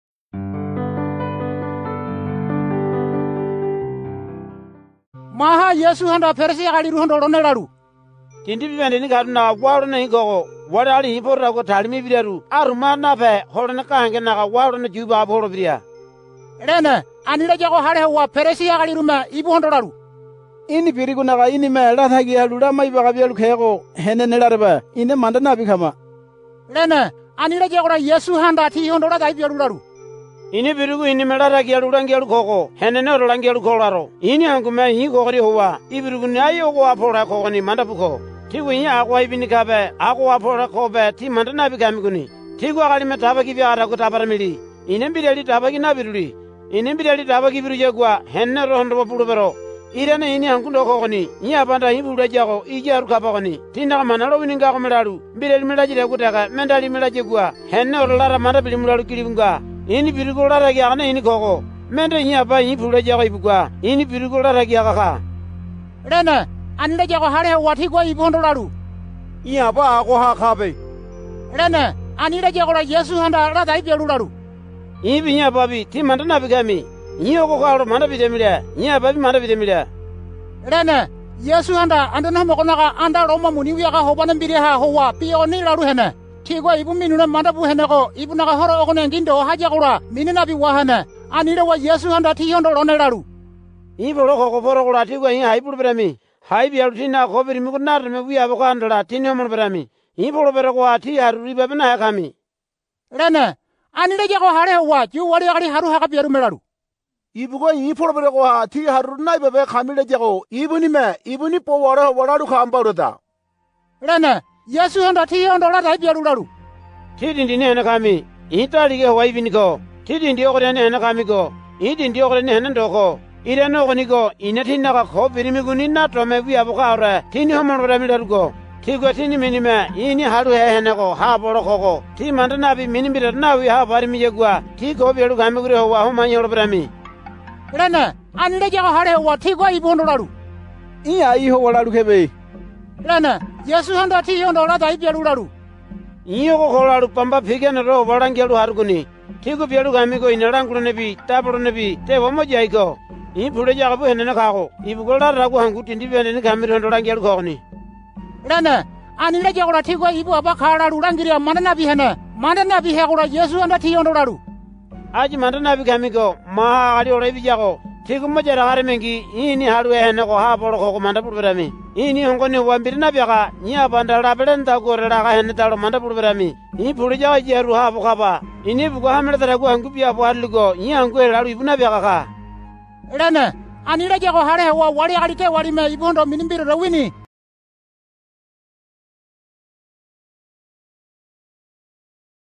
Huli - John 8:12-30 - Jesus is the light of the world [hui]
Scripture Videos from the Gospel of John in Huli of Papua New Guinea. [hui]